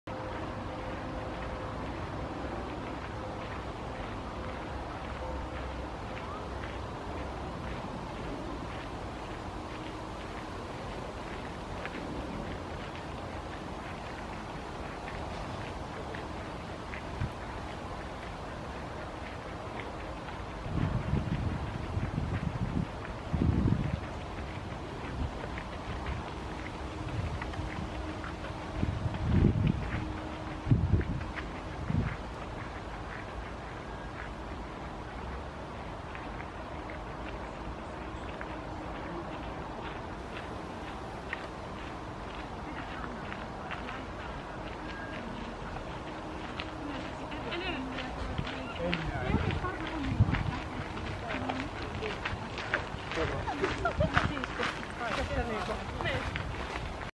Common Crane flock circling in a thermal
Common Crane (Grus grus) flock circling in a thermal to gain altitude.

Kurkiparvi (Grus grus) ottaa korkeutta nousevassa ilmavirtauksessa.